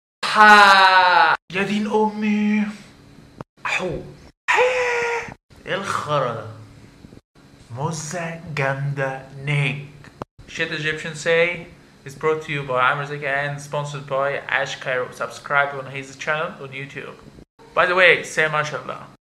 A7a Egyptians Say
Officially the most hilarious video ever of Egyptian people saying a7a.